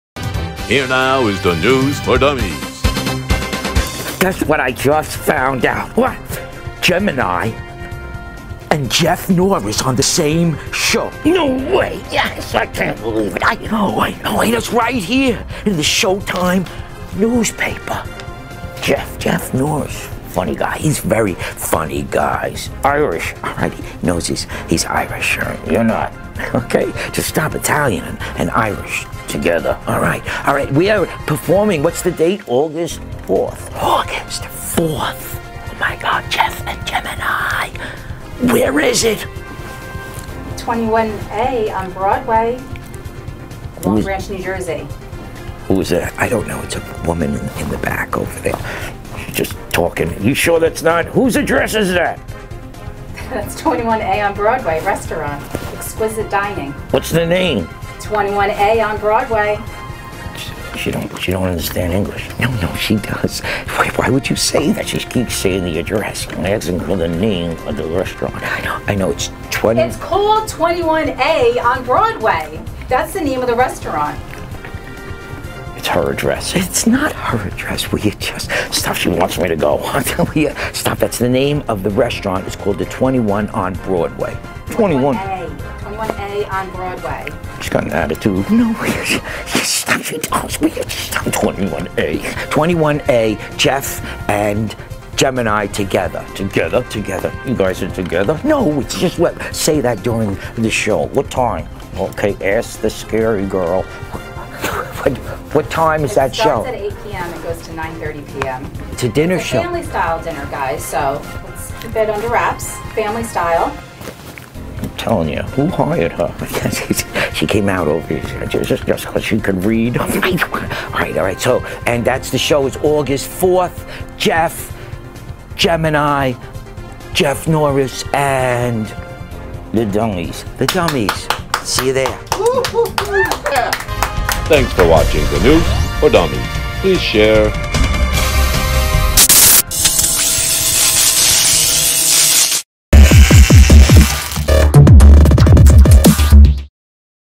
GEMINI & JEFF NORRIS AUG. 4 2022 EVENT AT 21A ON BROADWAY LONG BRANCH NJ. DINNER & SHOW !
GEMINI  & JEFF NORRIS AUGUST 4 EVENT AT 21A BROADWAY LONG BRANCH NJ.  DINNER AND A SHOW $75 FEATURED ON NEWS FOR DUMMIES.
Video Productions, Award Winning Videos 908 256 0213